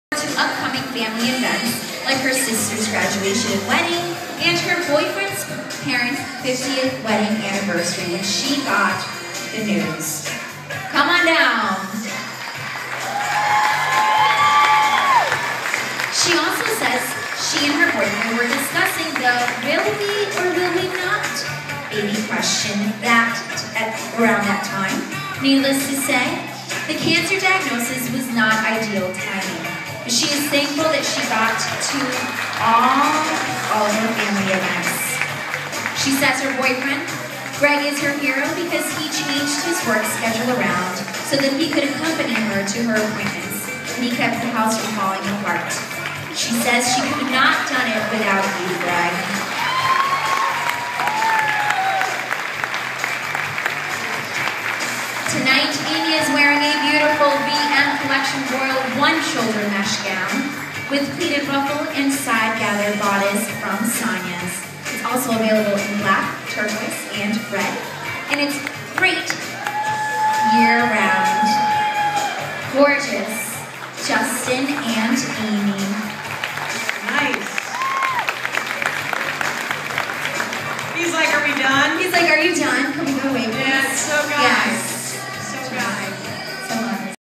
Breast Cancer Fashion Show
50' Closer founder and athlete Justin Sulham escorts a wonderful and inspiring breast cancer survivor down the runway at the 2013 Gloria Gemma Breast Cancer Foundation Survivors Fashion Show.